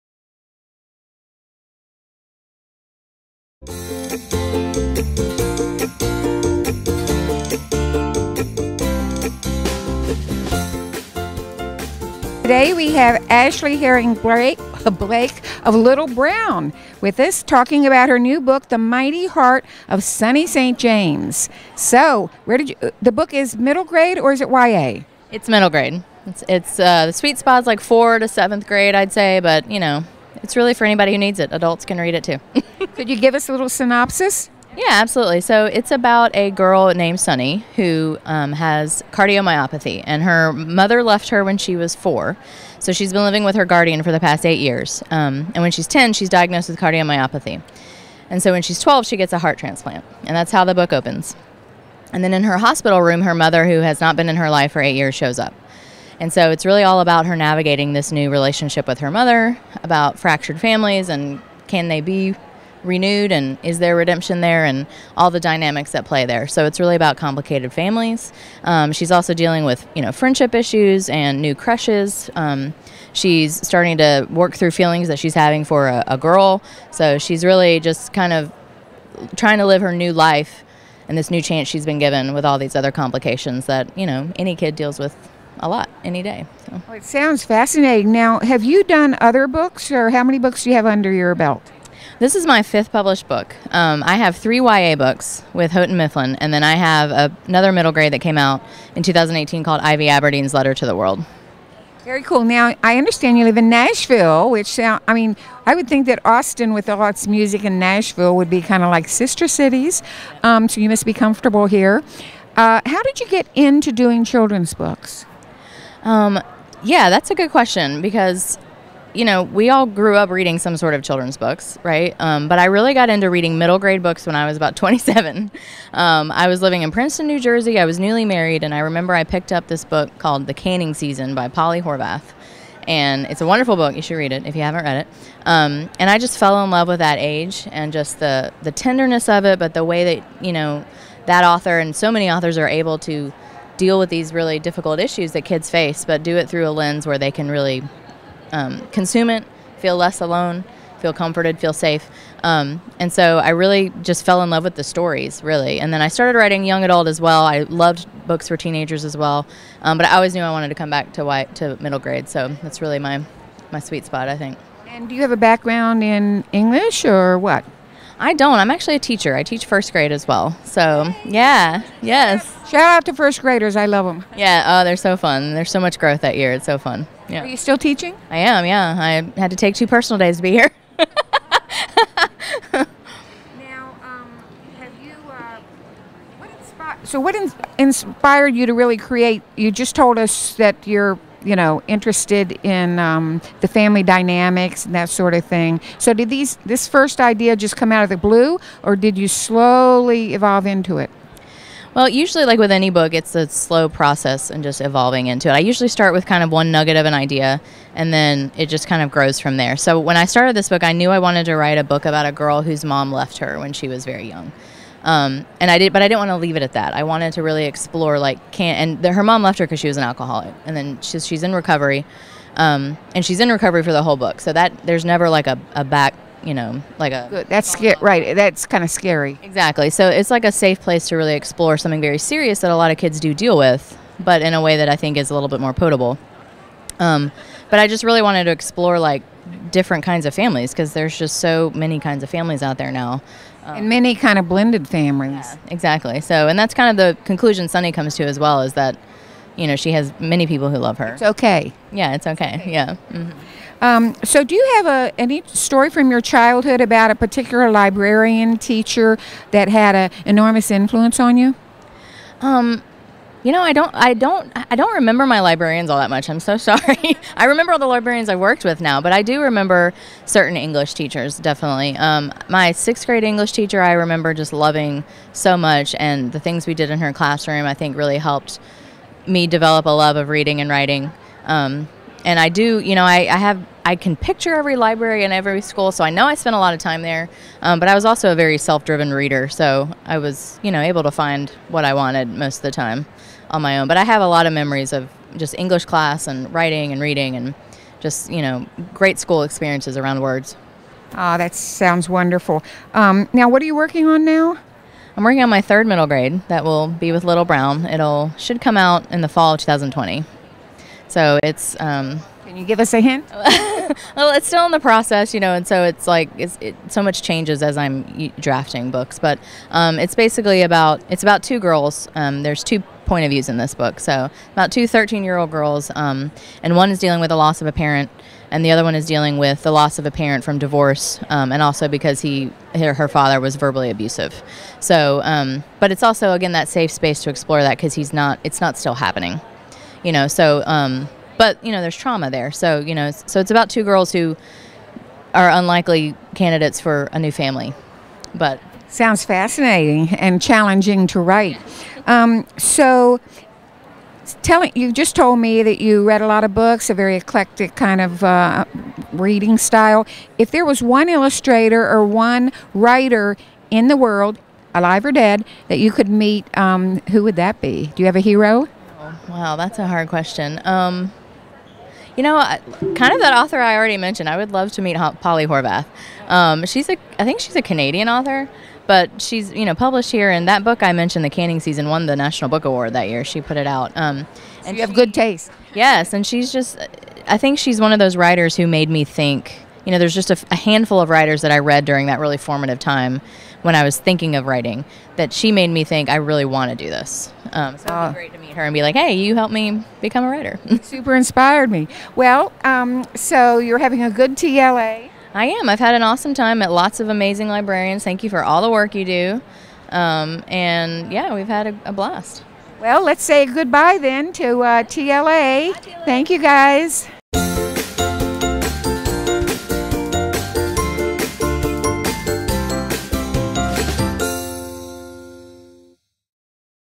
LIVE from TLA with Ashley Herring Blake THE MIGHTY HEART OF SUNNY ST. JAMES
KidLit TV LIVE is at TLA 2019!
Watch interviews with kid lit authors and illustrators
right here!
Visit us at TLA – KidLit TV Booth #2841

Schedule:
Wednesday, April 17

10:00 am CDT / 11:00 am EDT — Ashley Herring Blake –The Mighty Heart of Sunny St. James